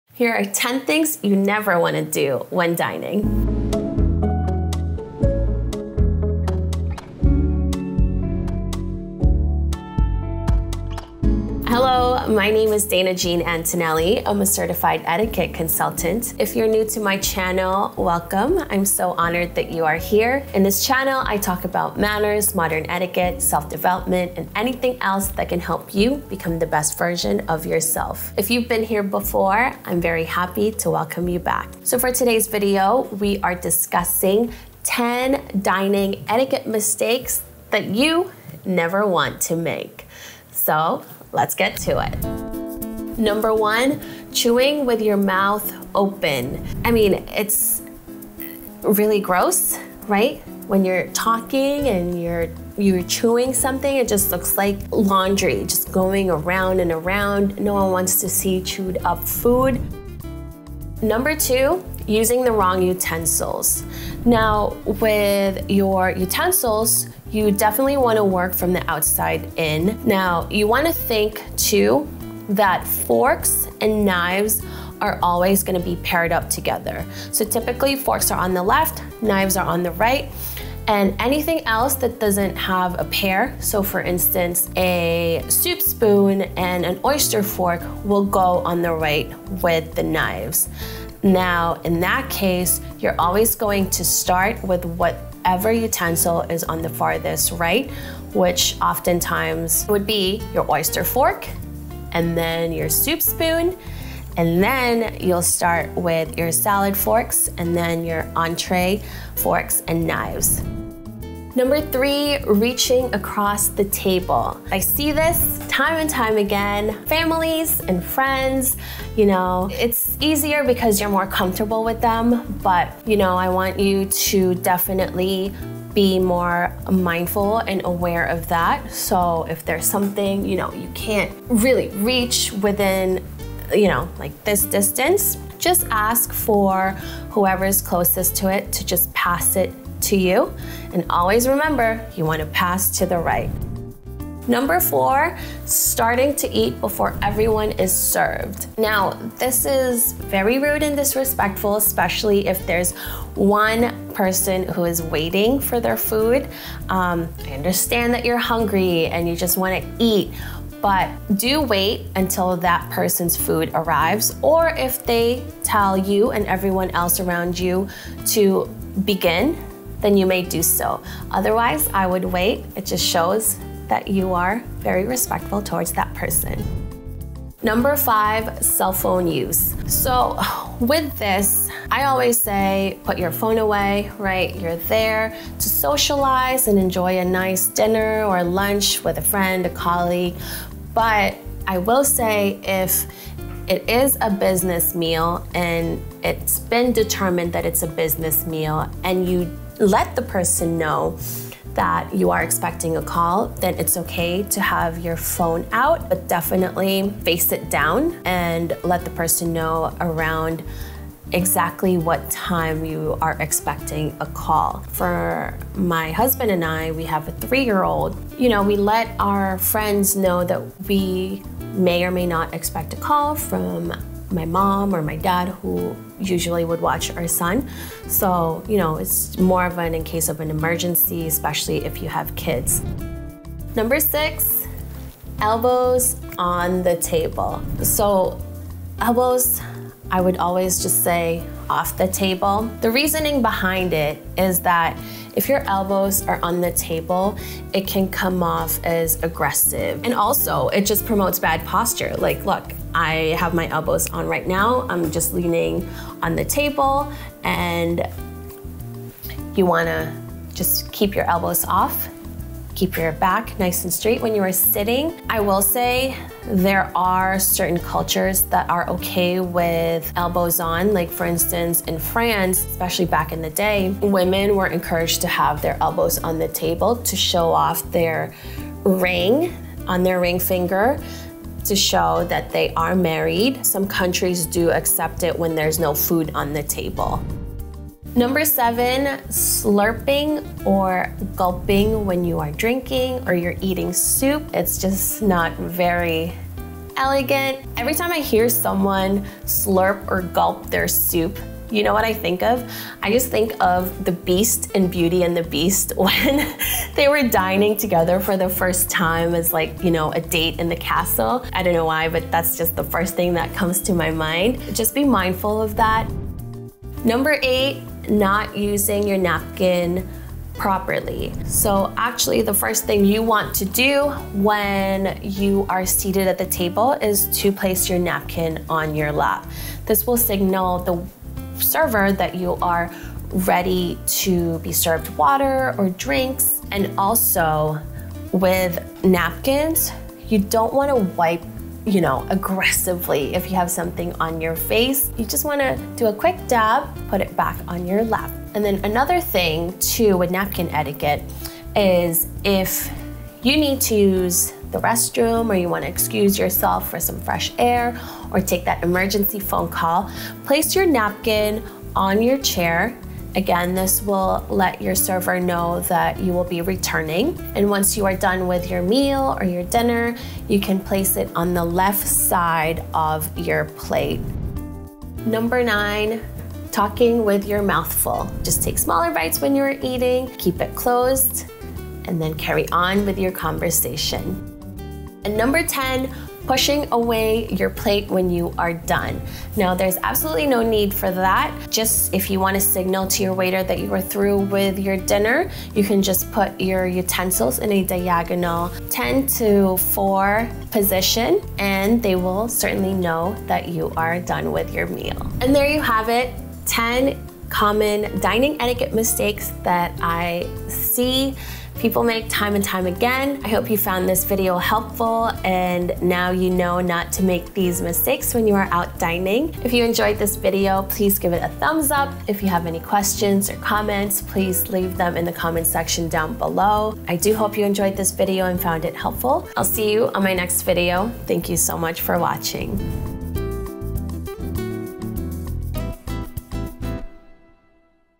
10 Common Dining Etiquette Mistakes
In this video, I go over 10 common dining etiquette mistakes that I see people do often. I hope you find this video helpful and you can avoid making these mistakes next time you dine out.

Remember, I'm on this journey with you on becoming the best version of yourself!